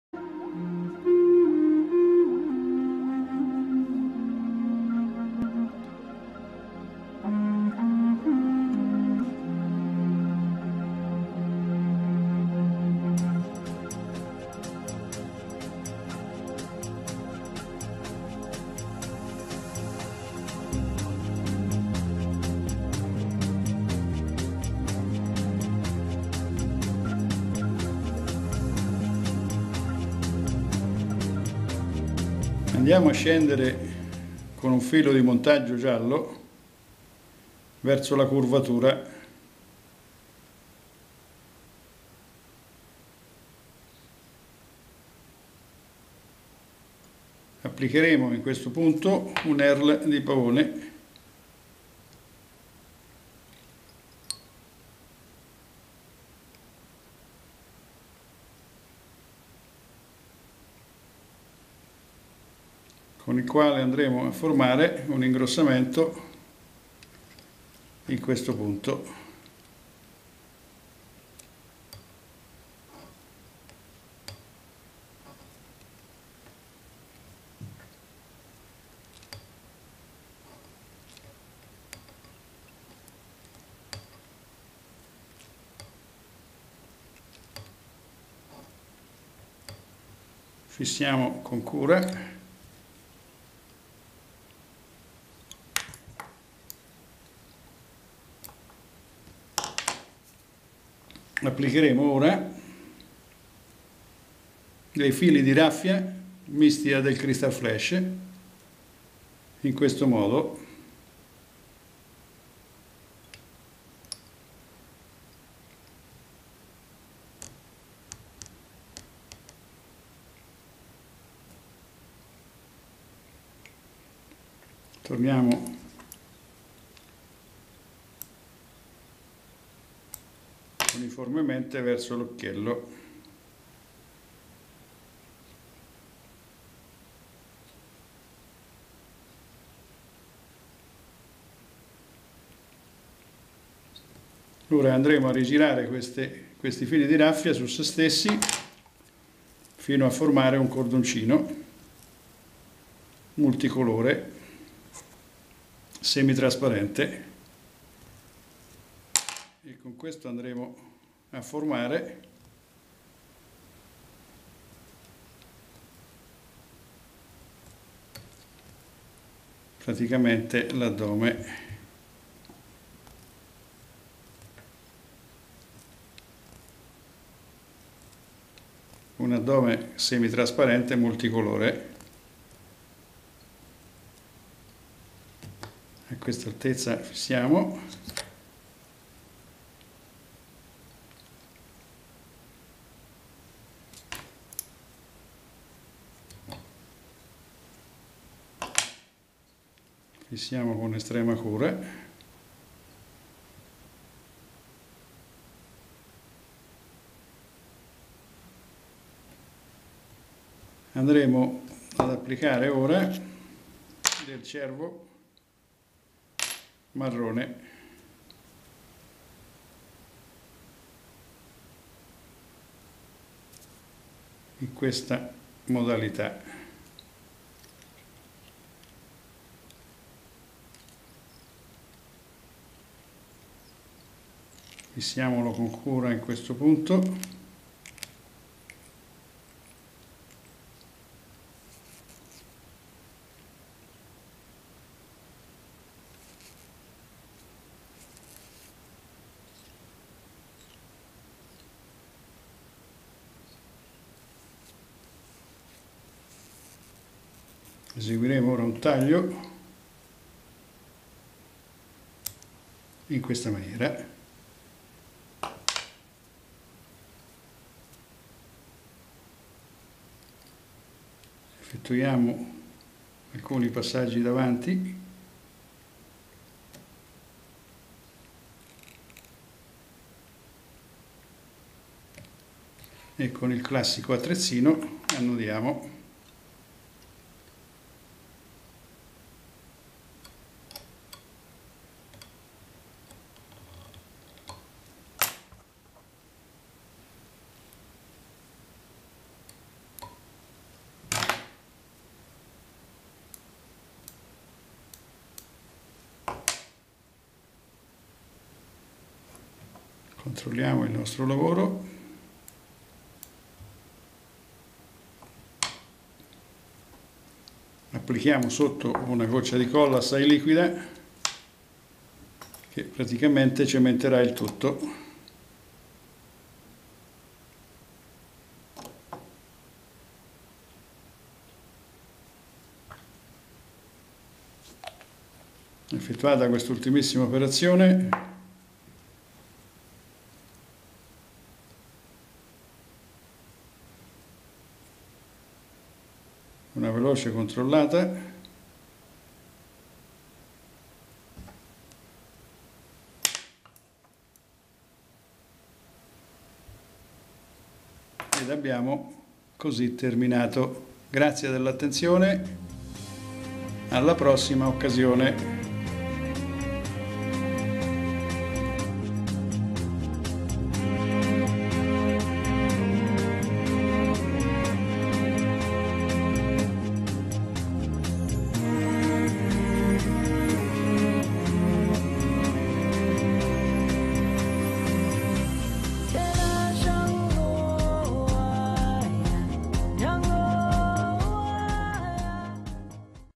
With Horns Emerging by Giancarlo
Il successo o meno di una battuta di pesca è strettamente collegata al comportamento che metteremo in atto una volta giunti nel luogo prescelto,  non vi è nulla di più errato e deleterio che montare  velocemente coda e finale e subito dopo iniziare a lanciare a destra e a sinistra alla spasmodica ricerca della prima cattura.

Ora vi spiego come affronto io una battuta di pesca; una volta giunto nel luogo prescelto monto con calma la mia attrezzatura, successivamente stiro il finale in modo da renderlo perfettamente rettilineo e ne controllo l'integrità ripristinando sempre la parte più debole il tip, che adeguerò in lunghezza e spessore alle caratteristiche del torrente, inizio poi con calma a guardarmi in torno cercando di cogliere informazioni per la successiva scelta  l'artificiale, tenendo conto dei livelli, del periodo stagionale, dell'andamento climatico dell'ultima settimana e del giorno stesso.
Eseguo poi pochissimi lanci di prova per testare che il tutto sia in perfetto ordine ed equilibrio, successivamente applicando tutte le strategie maturate in oltre cinquanta anni di pesca con la mosca inizierò la vera e propria battuta, limitando al minimo le presentazioni "poche ma buone", questa è la regola fondamentale per diventare dei veri e propri killer.

Limitazione di oggi va usata di regola "subito sopra subito sotto", il suo galleggiamento è  precario, ma altissima è la sua efficacia quando è semi sommersa.

MATERIALI: Ami grub leggeri 14/12/10. Addome formato da fili di rafia sintetica dorata e di krystal flash multicolore rigirati su se stessi. Ala cervo tinto di marrone. Filo di montaggio giallo limone.

MATERIALS: Hooks grub light 14/12/10. Abdomen, formed by strands of synthetic raffia golden and multicolored krystal flash turned over on themselves. Wing, deer dyed brown. Wire Assembly, lemon yellow.

A scuola di costruzione artificiali insieme a Giancarlo Donninelli, il (no-) Killer più spietato per le grandissime trote ITALIANE